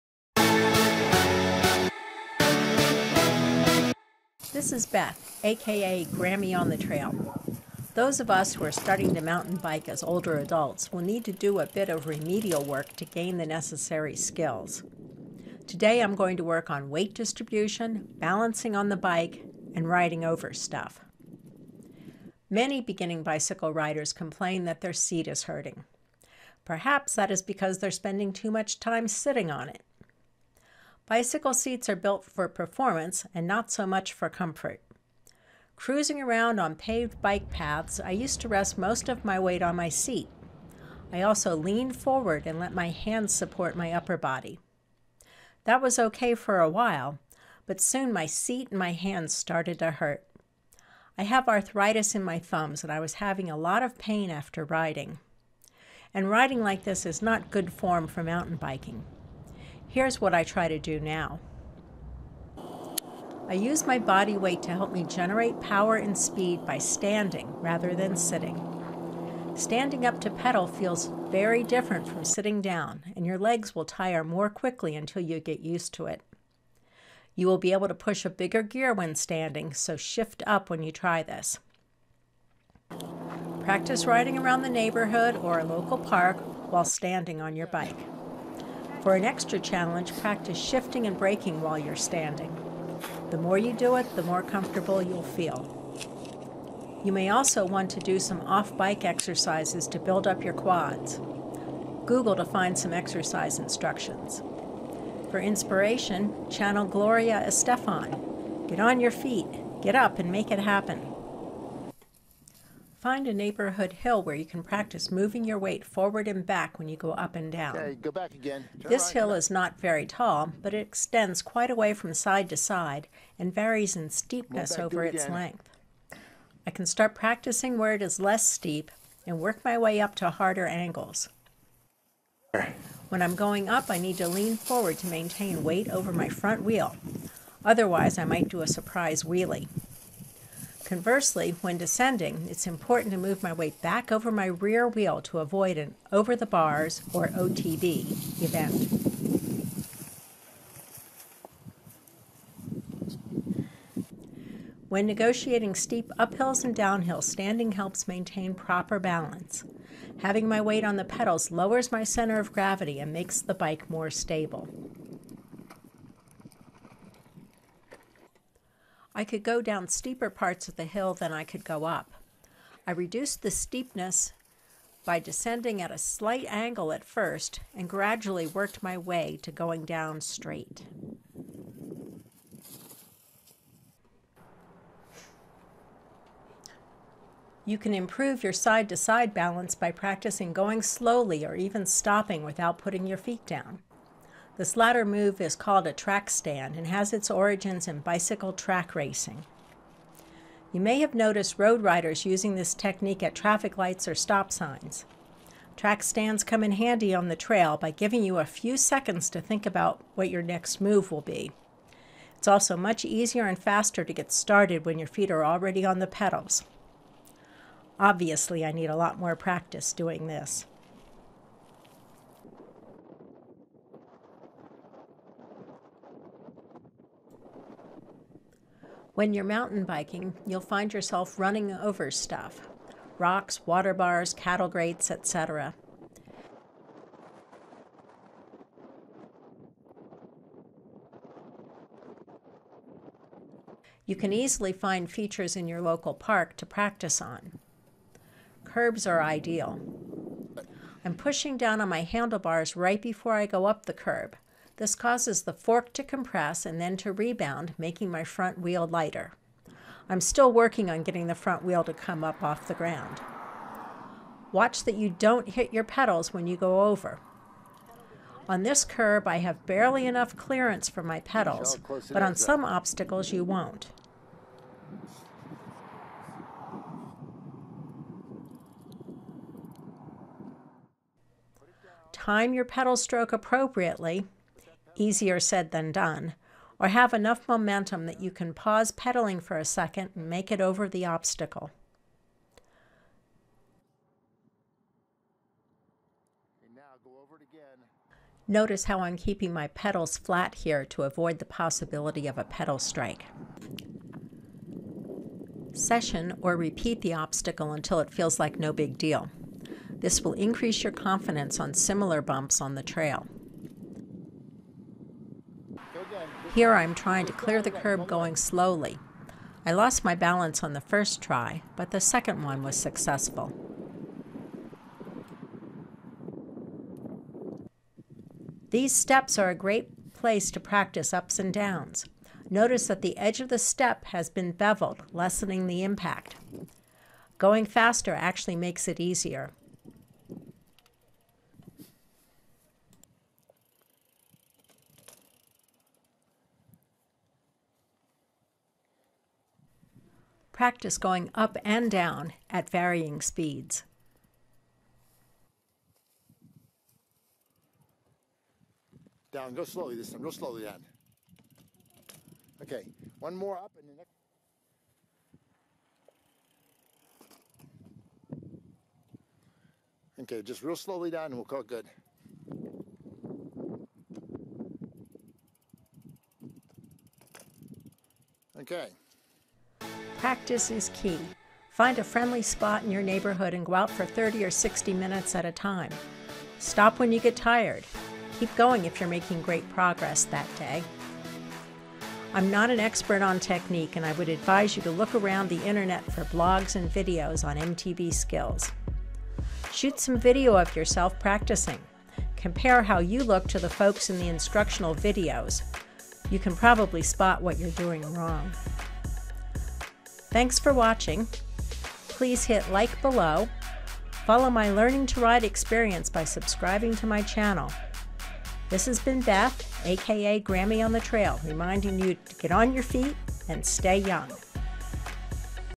More Beginner Skills
Watch as I practice some more beginner skills: weight distribution on the bike, balancing, and riding over stuff.